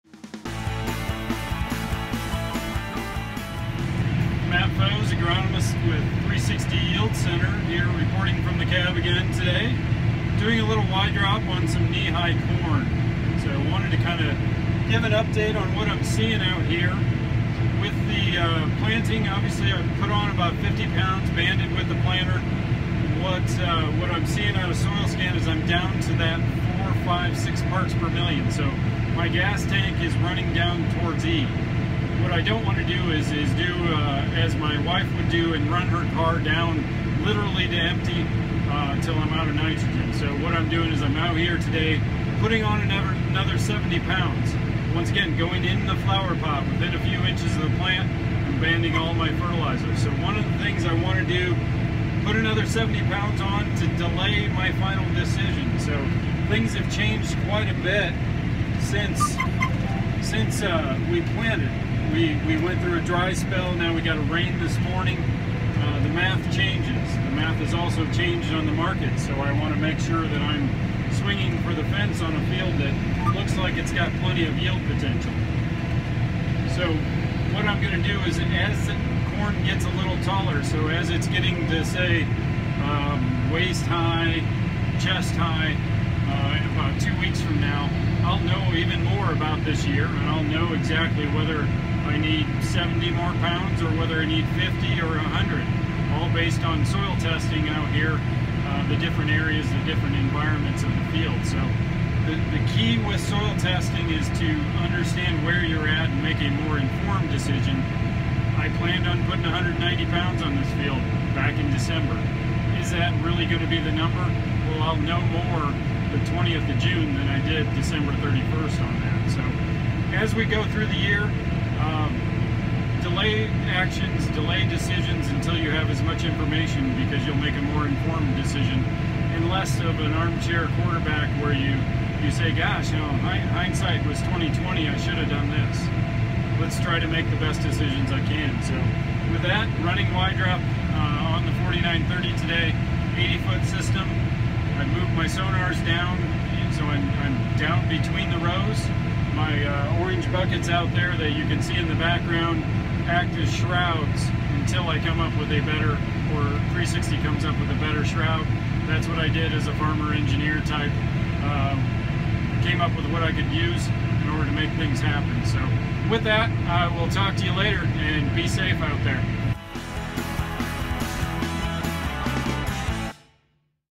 360 Yield Minute - 360 Y-DROP - Matt Foes
While running 360 Y-DROP in knee-high corn, Matt Foes, Regional Agronomy Manger with 360 Yield Center, explains why it's important to not let the corn's nitrogen supply run all the way down to empty.